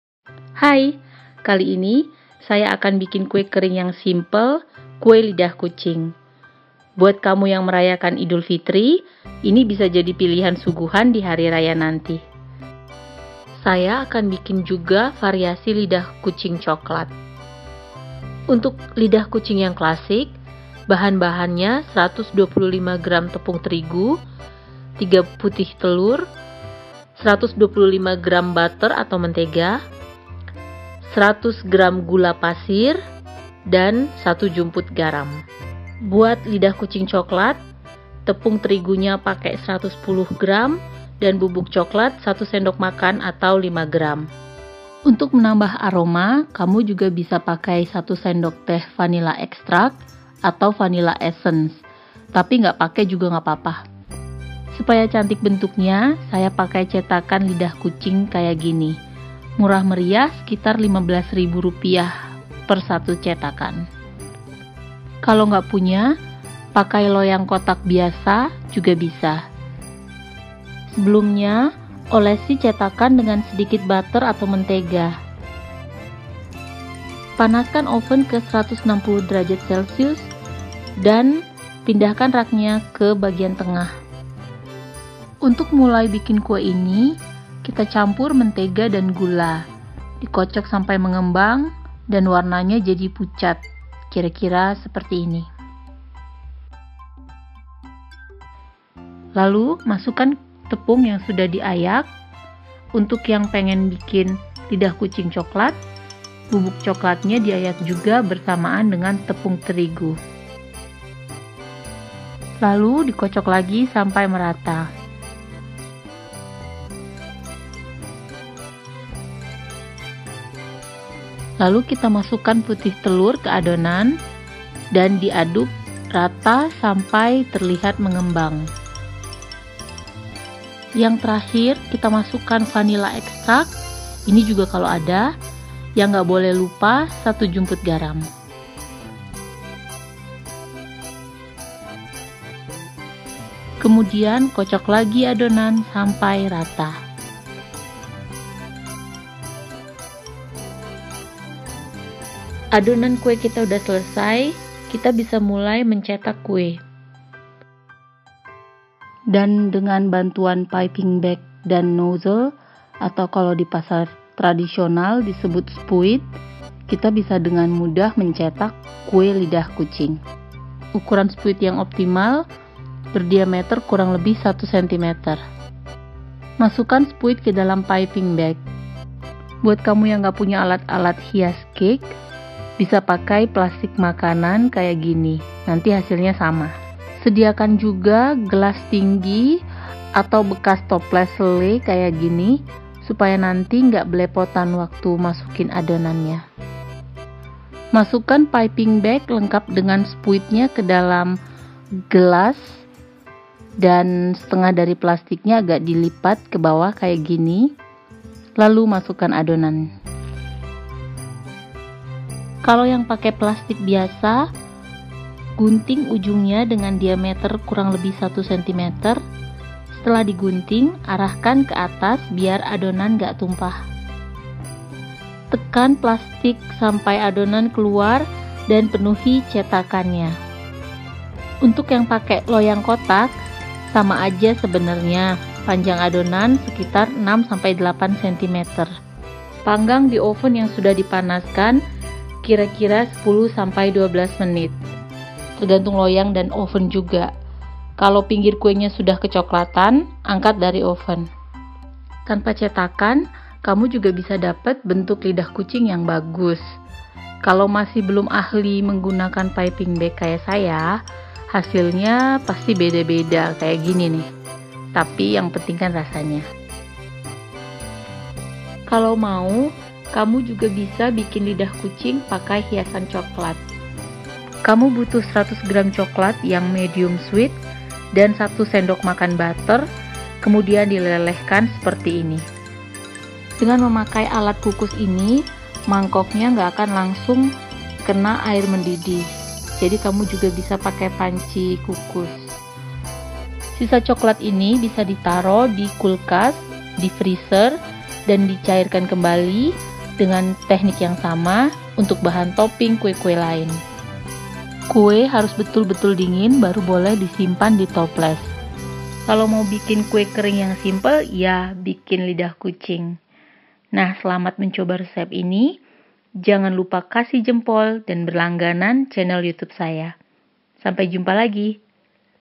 (Eng Sub) Resep Kue Lidah Kucing (Langues De Chat/Cat Tongue Biscuits) RESEP KUE KERING
Kue lidah kucing yang bisa jadi pilihan suguhan di hari raya Idul Fitri. Bikinnya gak ribet dan bahannya pun murah meriah :) Renyah nya dan manisnya kue kering yang satu ini, bikin ketagihan.